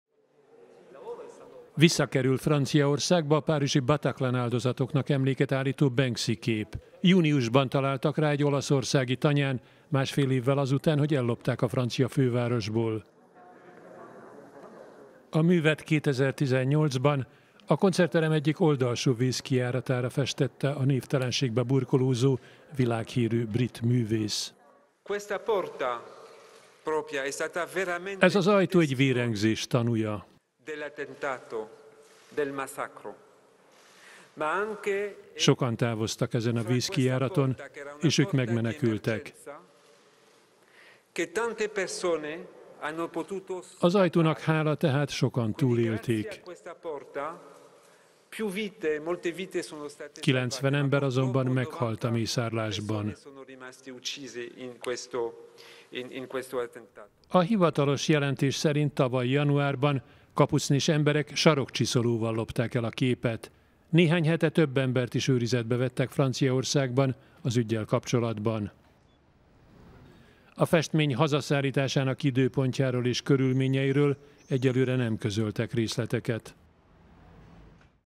Hazakerül a Bataclan-áldozatoknak emléket állító, ellopott Banksy-kép
A párizsi Bataclan-áldozatoknak emléket állító Banksy-képet hazatérése előtt ideiglenesen kiállítják Rómában. Júniusban találtak rá egy olaszországi tanyán.